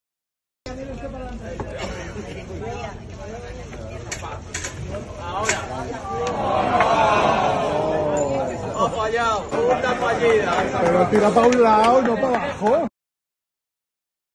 Almeida vuelve a poner la nota cómica en la inauguración de la placa en homenaje a Casa Lúcio
El alcalde de Madrid pone la nota cómica en la inauguración de la placa en homenaje a Casa Lúcio.